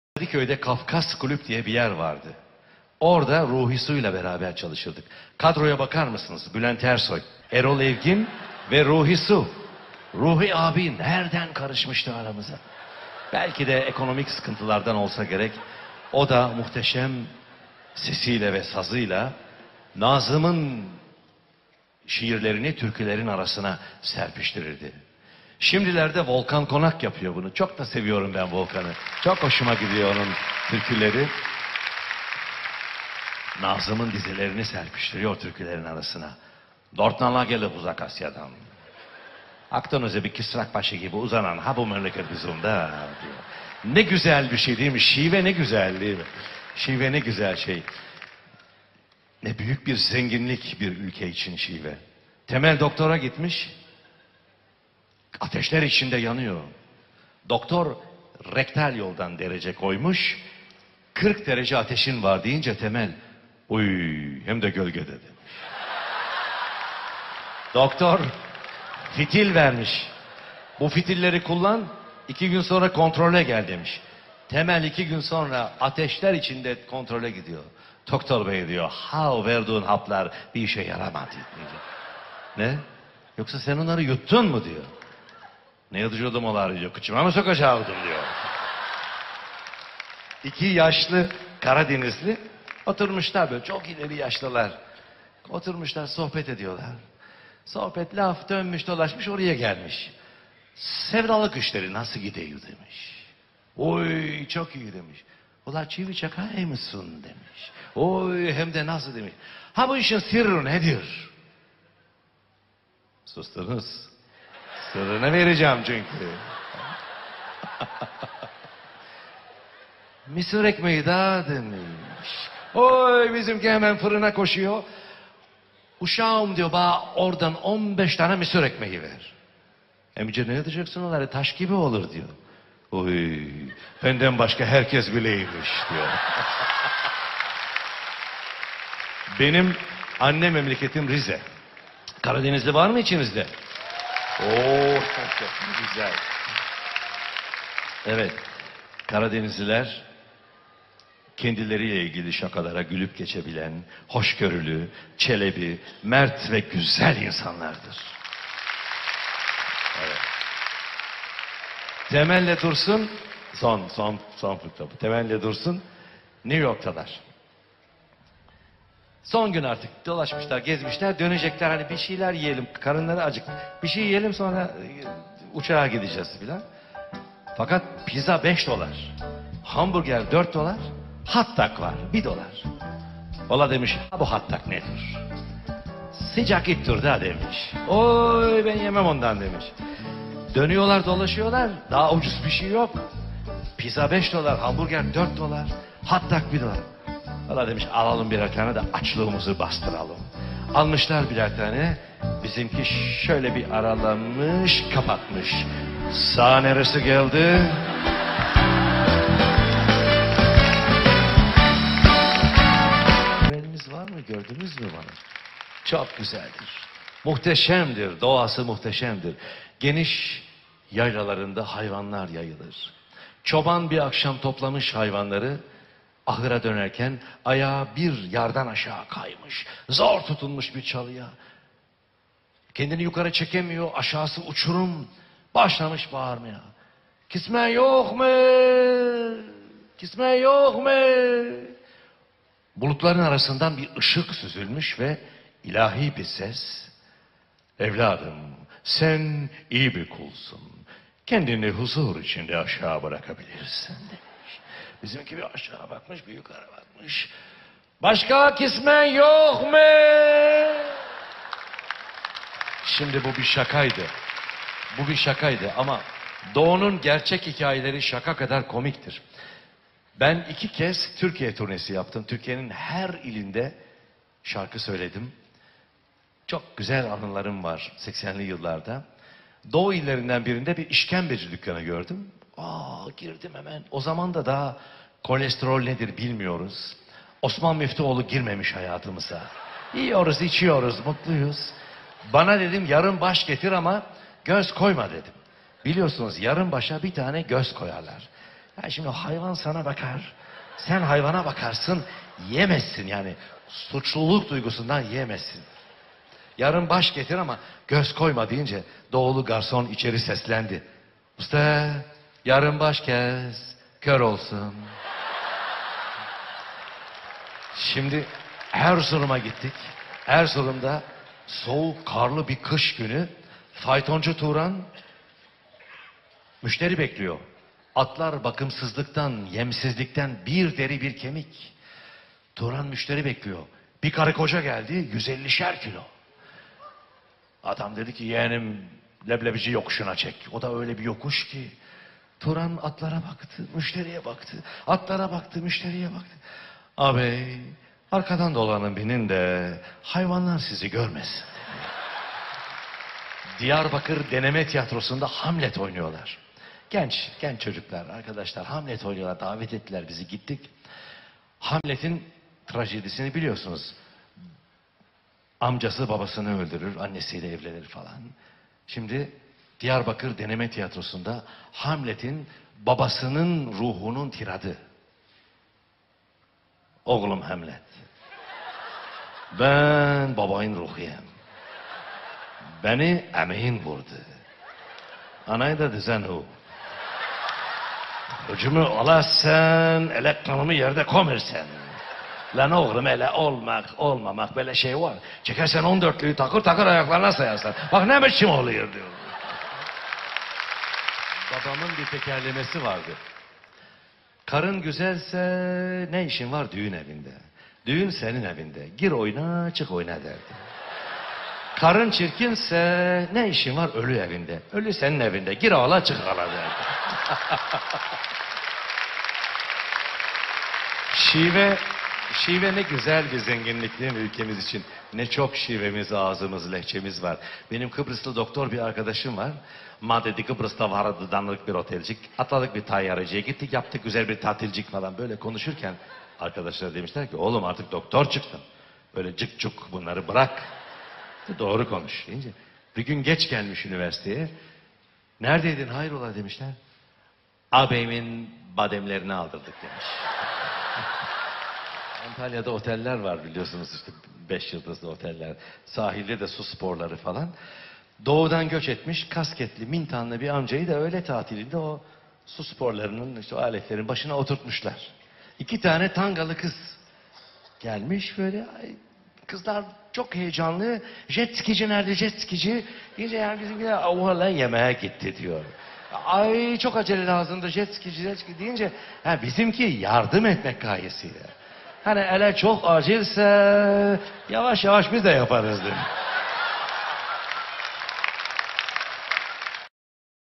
Erol Evgin Fıkraları ve Anıları
Diyarbakır'da Hamlet Oynarsa :)

Erol Evgin'in Anlatımıyla Komik Fıkralar ve Kendi Anıları